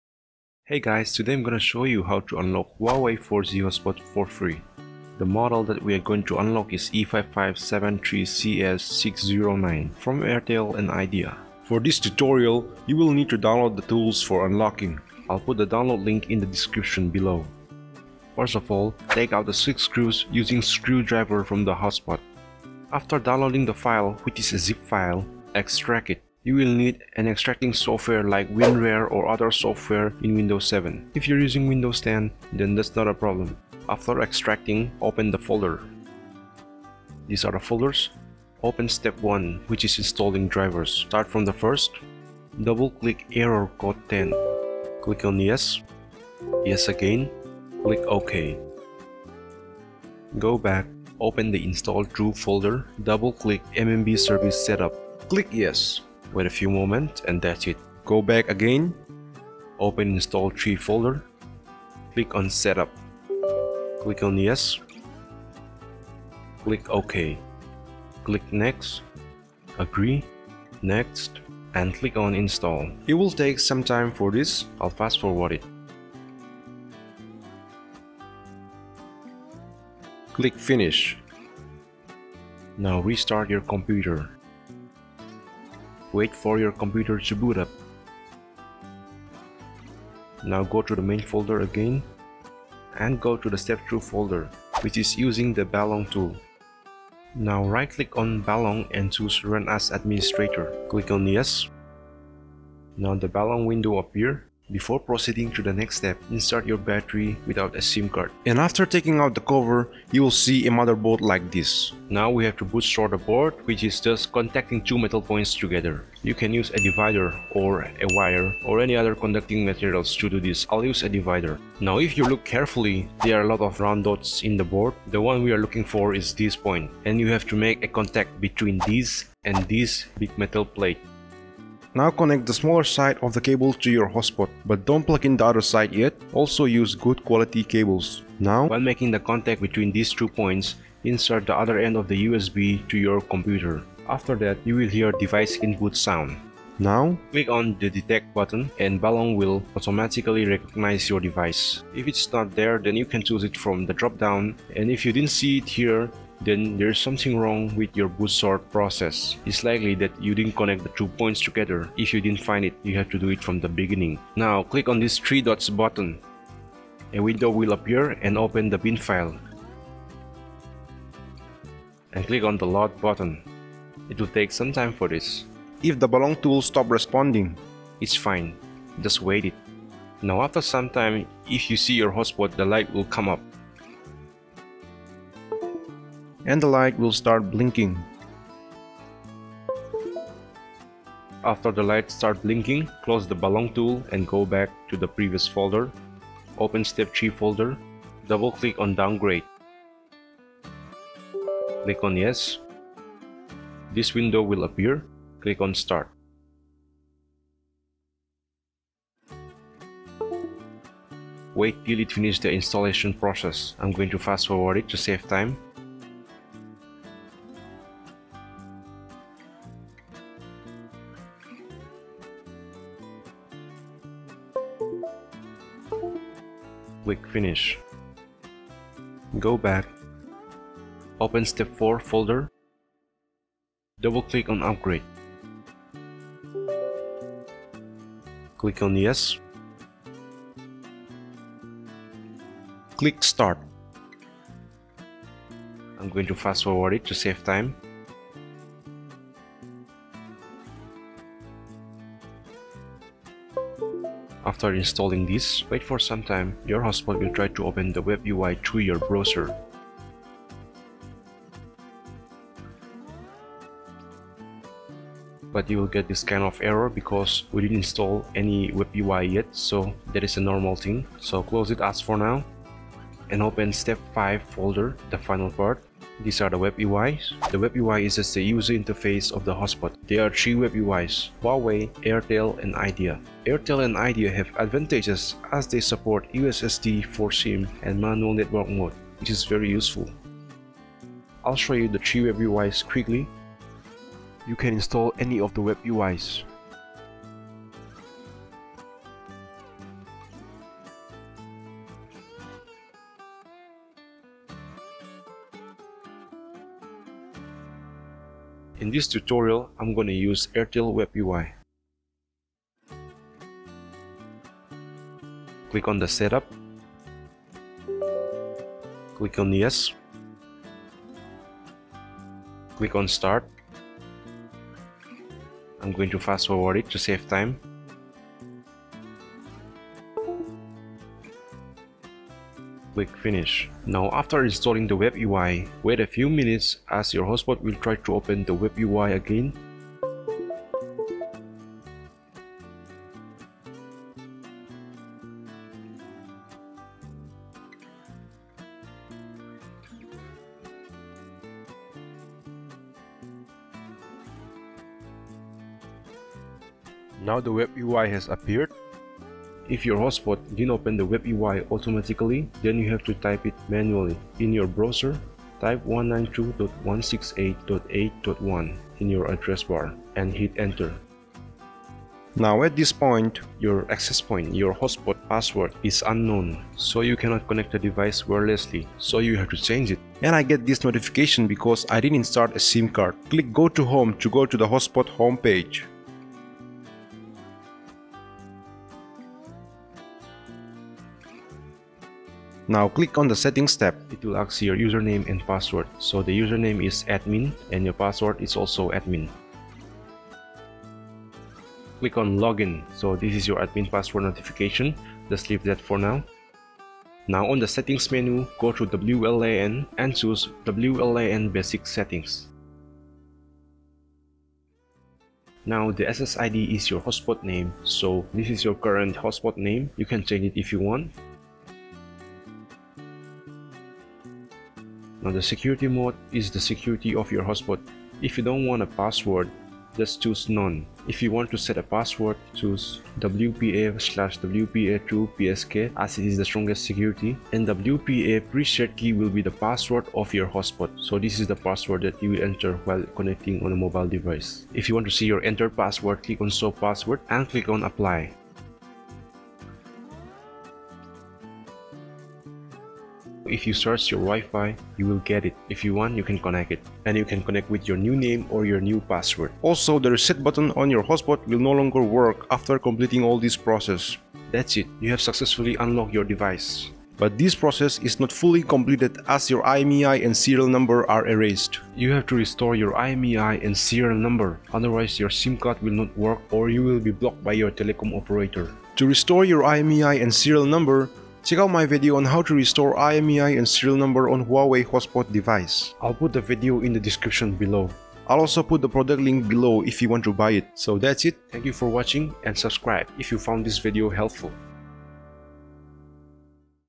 How To Use Any SIM Card On Airtel & Idea Hotspot (Unlocking) For Free 2018 New Method
YOUR IMEI & SERIAL NUMBER ARE ERASED IN THIS PROCESS SO YOU MUST RESTORE YOUR IMEI & SERIAL OTHERWISE YOUR HOTSPOT WILL NOT WORK WITH SIM CARD
(It is illegal to have incorrect IMEI & Serial)

NOTE TO AVOID ERROR CODES :
* YOU MUST RESTORE YOUR IMEI & SERIAL NUMBER OTHERWISE YOUR HOTSPOT WILL NOT WORK WITH SIM CARD
* STARTING FROM STEP 3 TO STEP 5  WAIT AT LEAST 1 MIN AFTER FINISHING EACH STEPS TO AVOID RANDOM ERROR
* DO NOT INSERT YOUR SIM CARD WHILE FOLLOWING THIS VIDEO
* UNPLUG ANY UNUSED EXTERNAL DEVICE TO AVOID ERRORS